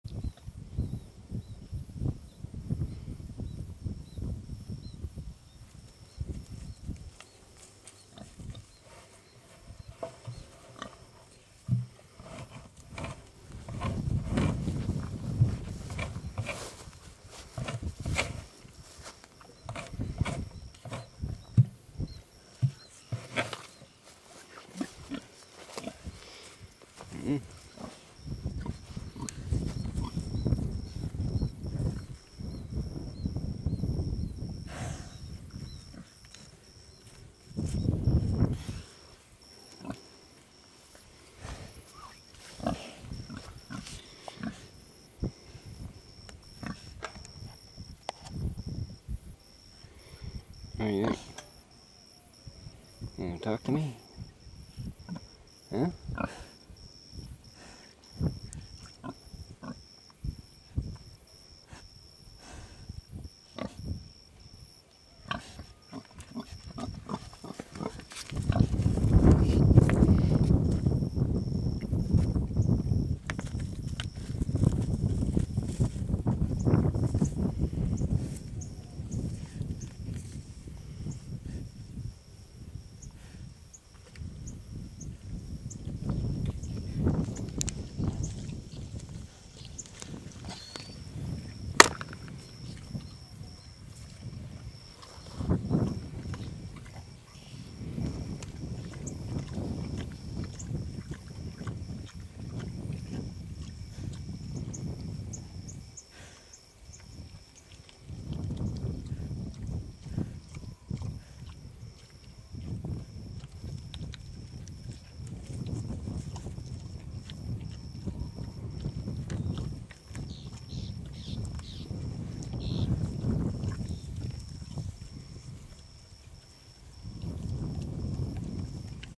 Red Wattle
This is our red wattle sow. Little Red Girl, she's not so little anymore... Most of the noise you are hearing is thunder, not so much wind.